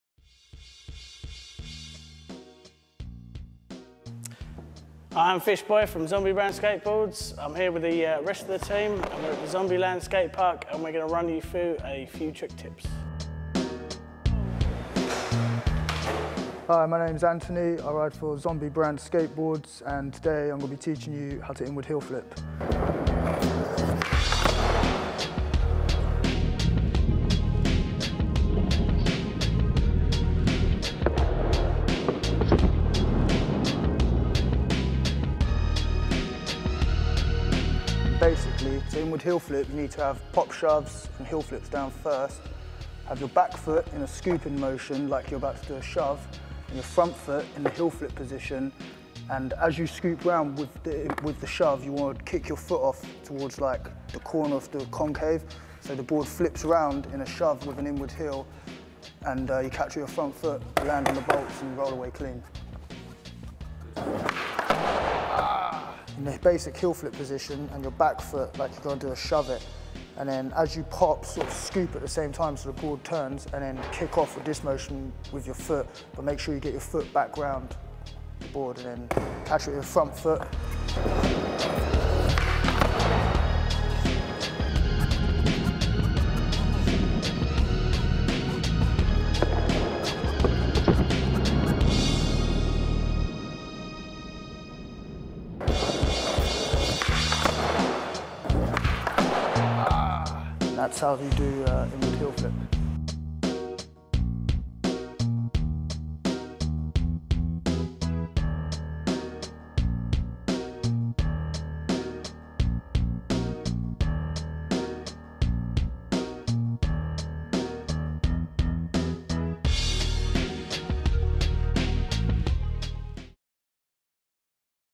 How To Do An Inward Heelflip Trick
This guide shows you How To Do A Inward Heelflip